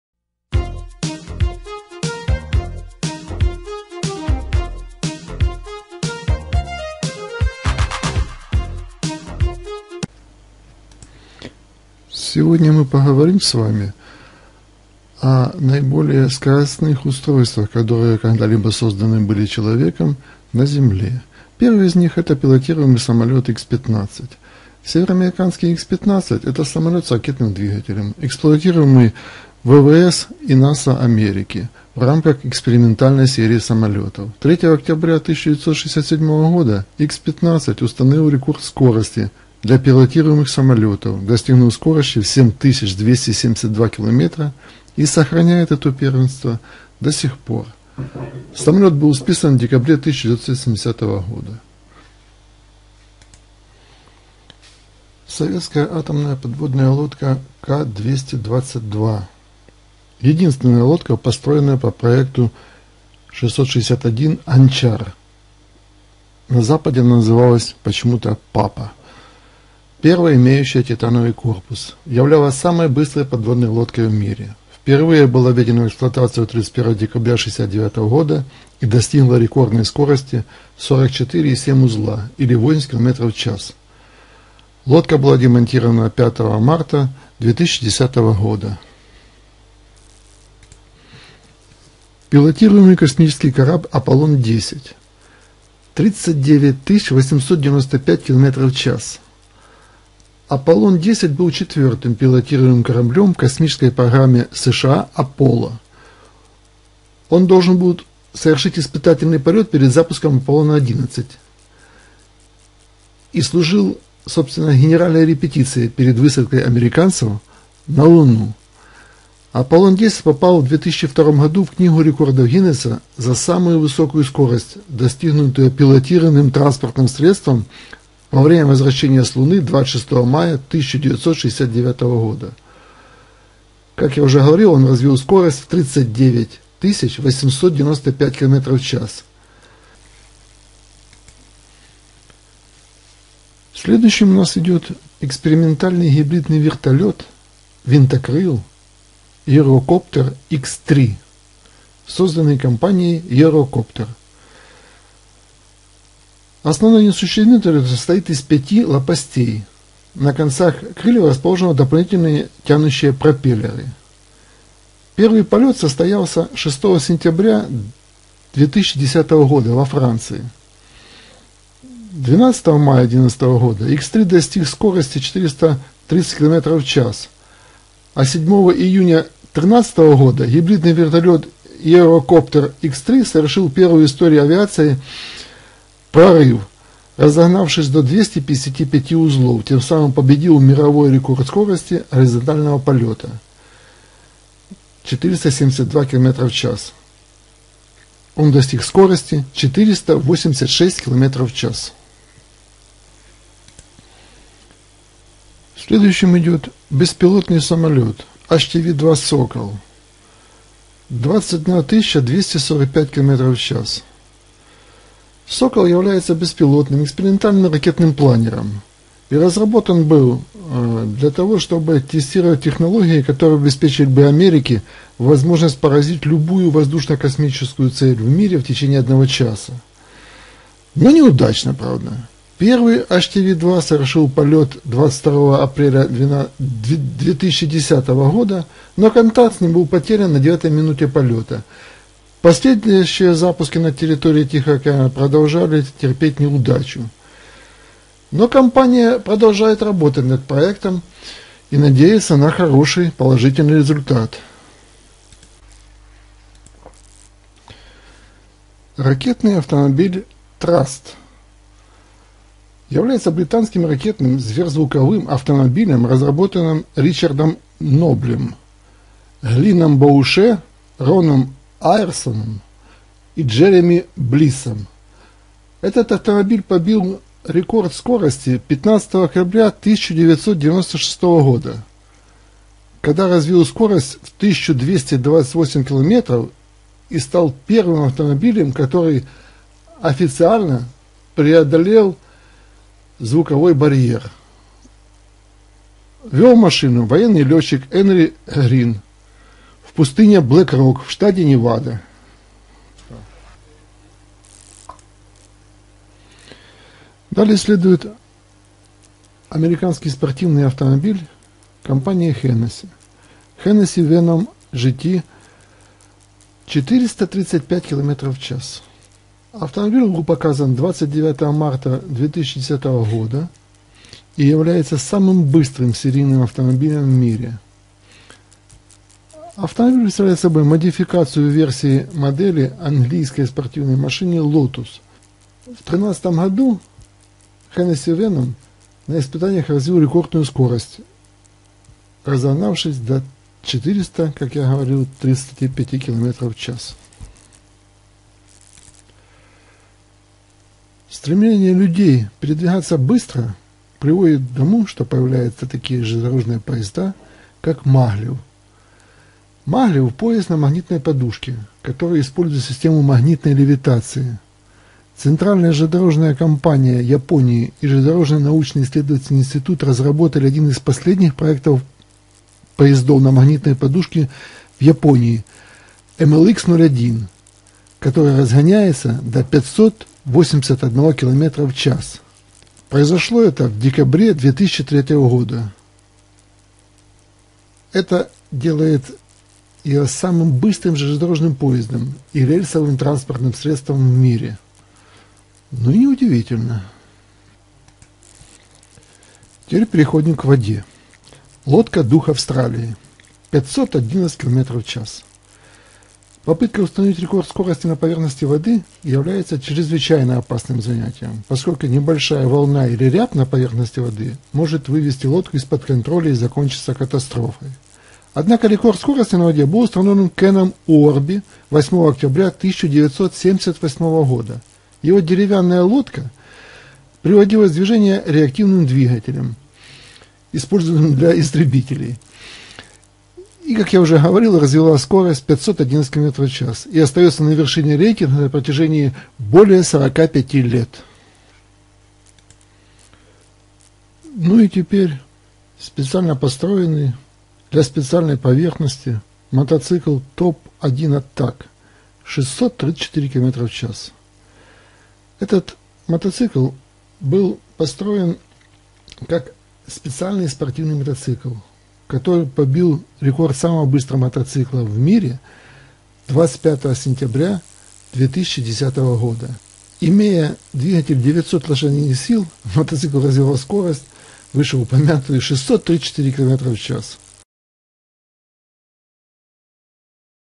самые быстрые на земле
Самые быстрые транспортные средства созданные людьми.
Гениальные технологии и изобретения на службе у человека.
Десять устройств передвигающихся по земле, воде и воздуху.